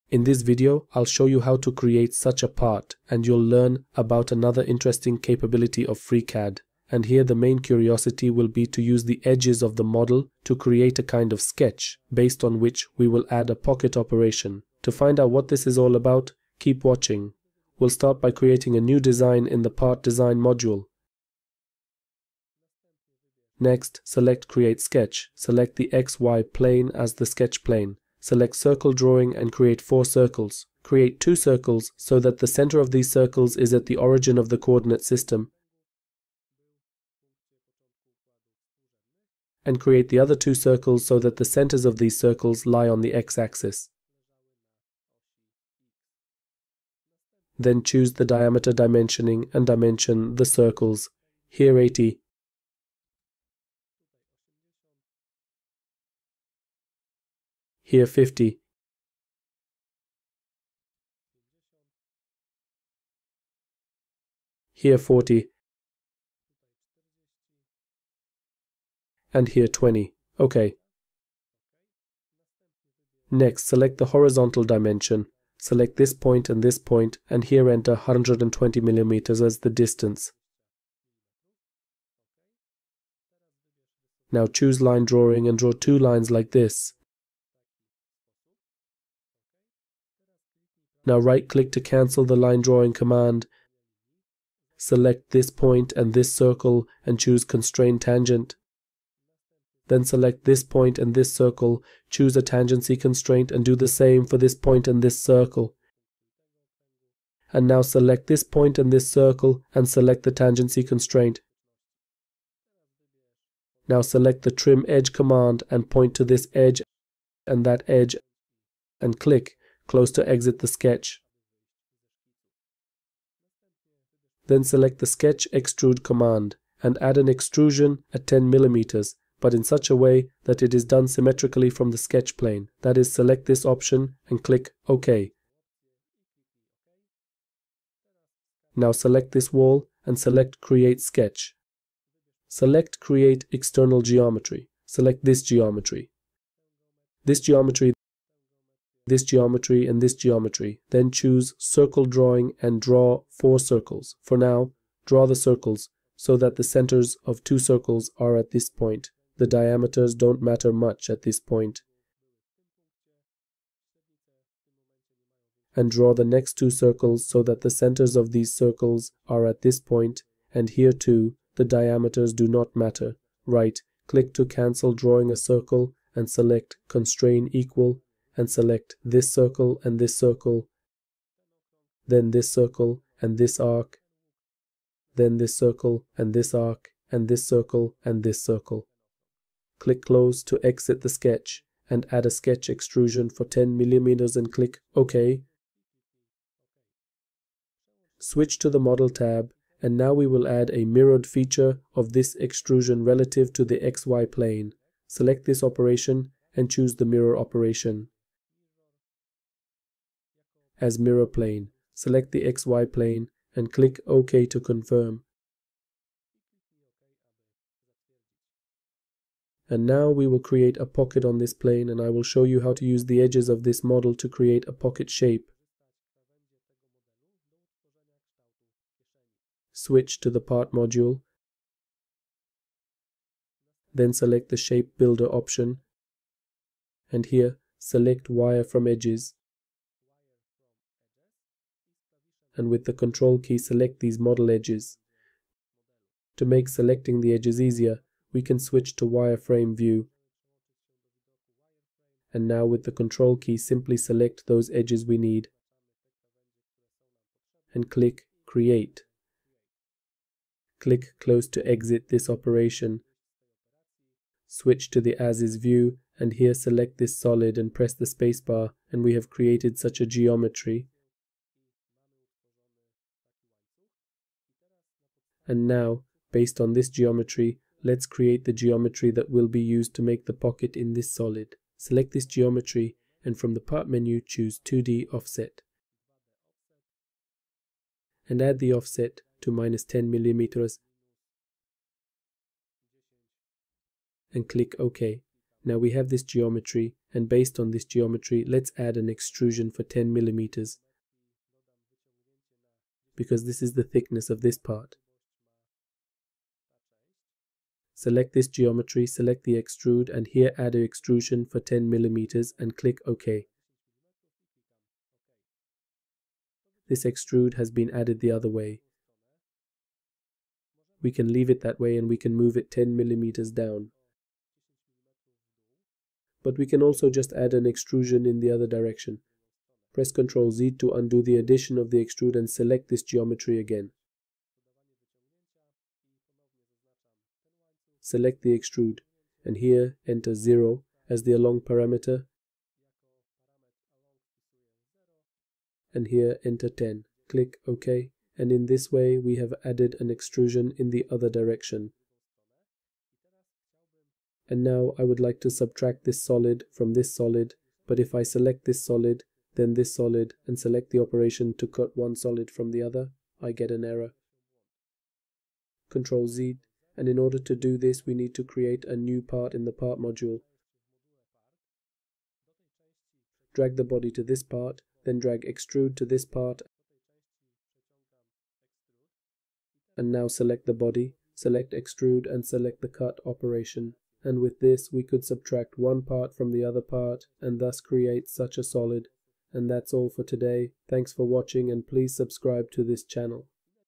FreeCAD Tutorial - Convert Model Edges to 2D Geometry
In this lesson we will design a sample part and the goal of this lesson is to create 2D geometry from the edges of the model. And then we will create an offset of this geometry and create a pocket in the model.